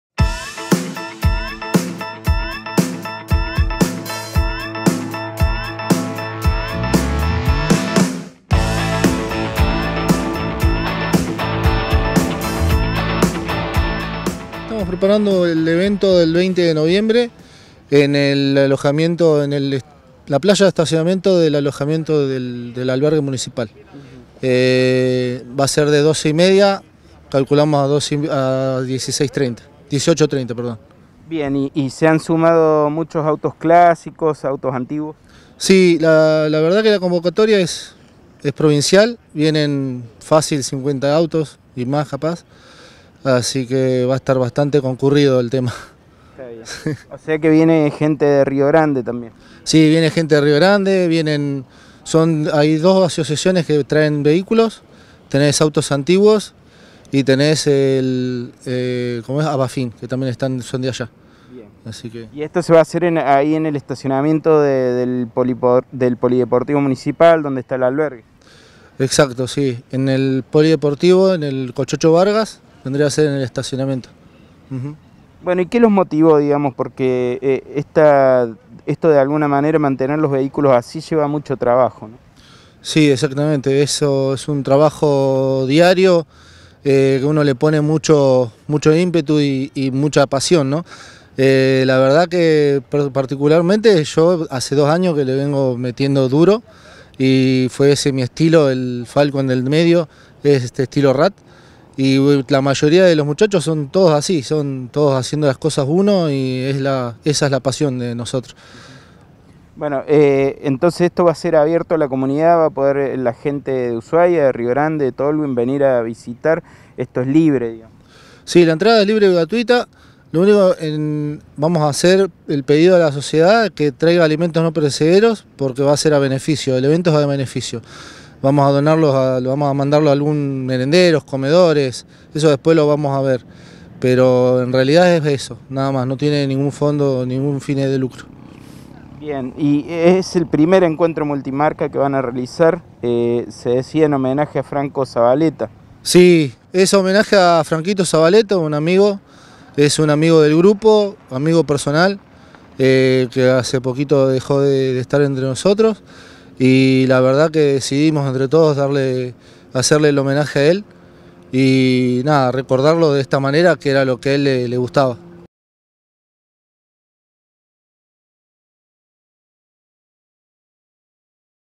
SE VIENE EL 1ER ENCUENTRO MULTIMARCA DE AUTOS CLÁSICOS
Se realizará en Ushuaia el próximo sábado 20 de noviembre, asistentes podrán llevar alimentos no perecederos para comedores.